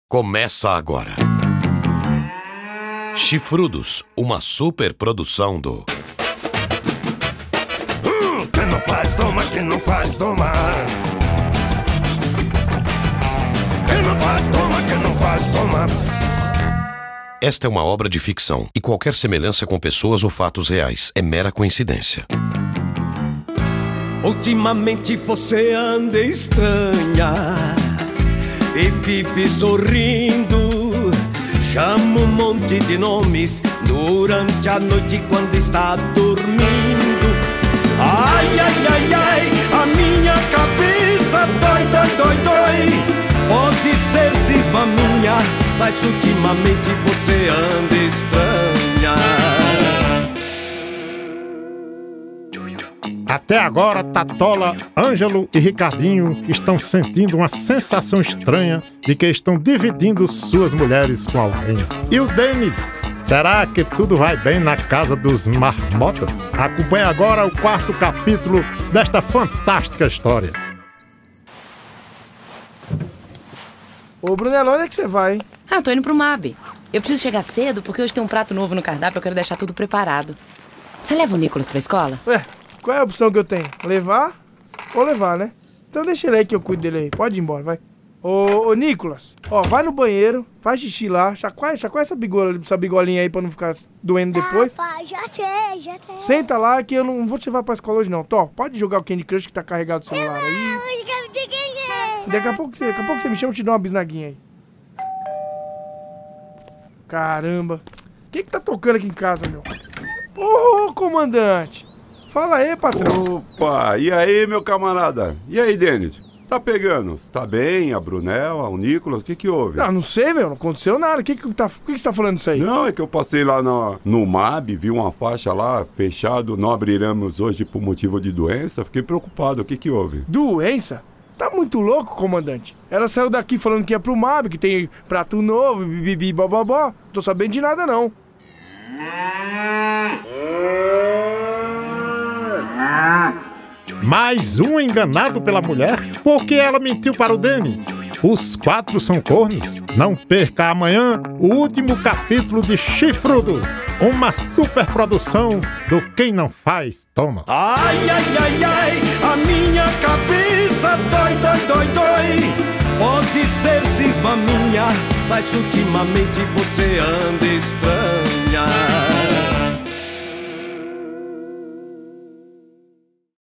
Chifrudos - Episódio 4
Quarto Episódio da Rádio novela Chifrudos. Mais uma super produção Quem não faz Toma!